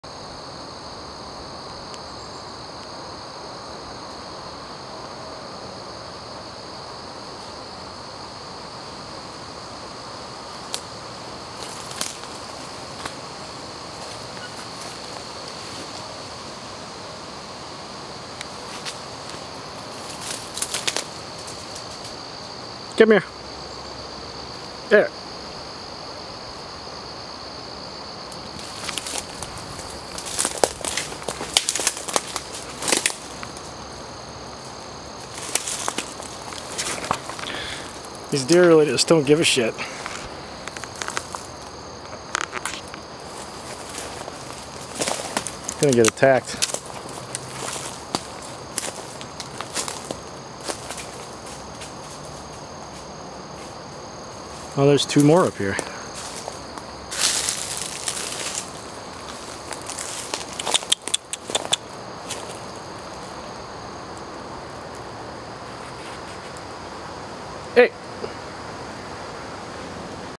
Deer Just Not Giving a Shit
Domestic deer in a forest that's surrounded by houses. Not too surprised they let me walk to about 10' from the three of them. The DNR just hired"marksmen" in the spring to reduce this herd, because they were eating too many flowers and rare plants.